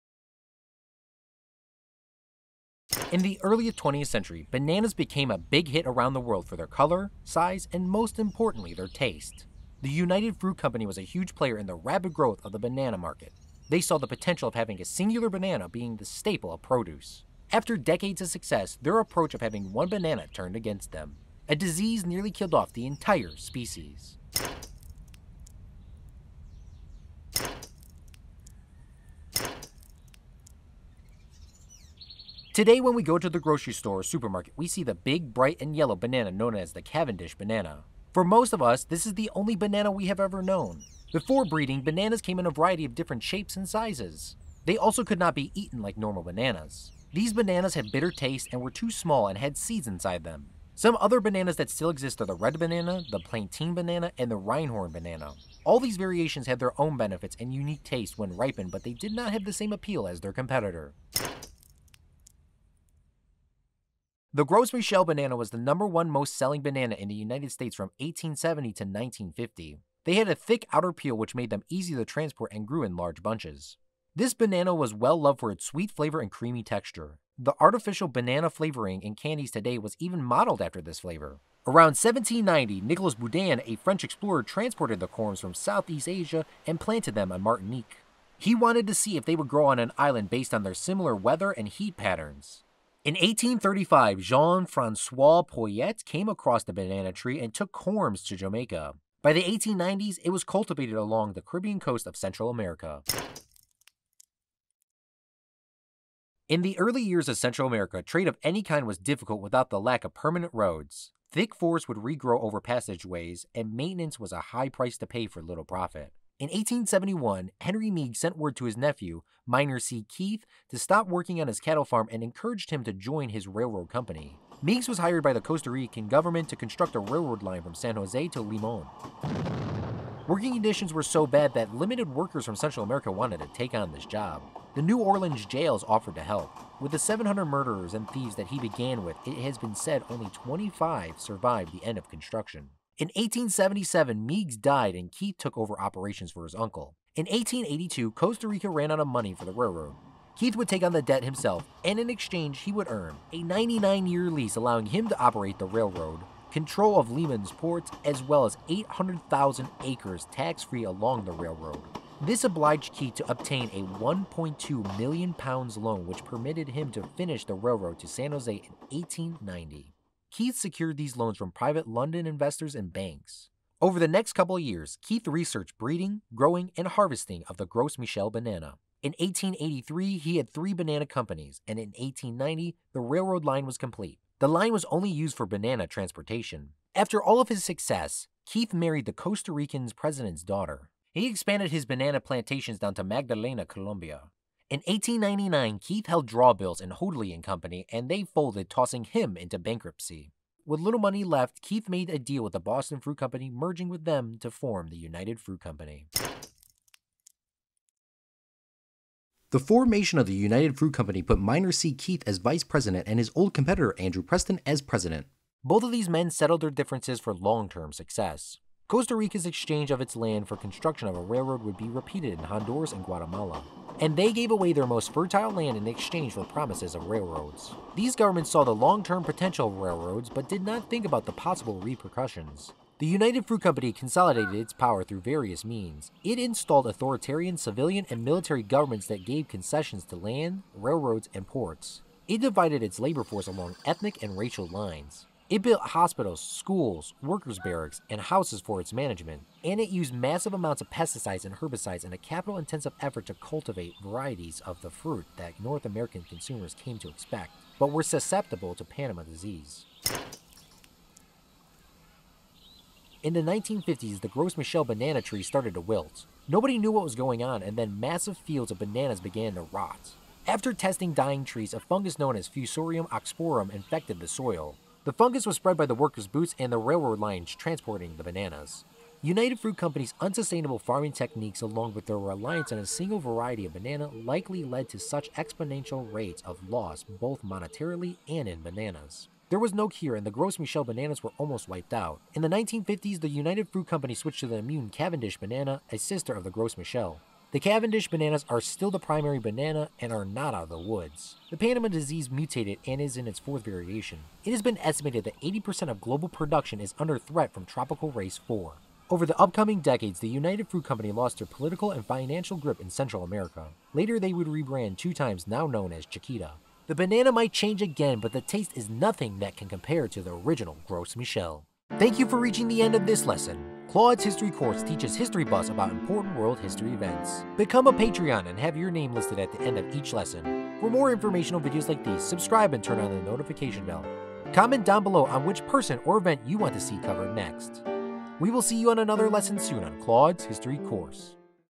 Original Banana and United Fruit Company | CHC
In the early 20th century bananas became a big hit around the world for their color, size and most importantly their taste. The United Fruit Company was a huge player in the rapid growth of the banana market. They saw the potential of having a singular banana being the staple of produce. After decades of success, their approach of having one banana turned against them. A disease nearly killed the entire species off.

0:00 Intro
0:29 Types of Bananas
1:01 Gros Michel
1:43 Minor C. Keith
3:26 United Fruit Company
4:20 Panama Disease
5:21 Outro

Baker Library Special Collections,
Harvard Business School

Castle Films,
Institutional Cinema Service. Inc

The William J. Ganz Co.,
 United Fruit Company

Beluga, Caroline. (2023).

Australian Government. Department of Agriculture, Fisheries and Forestry. (2023).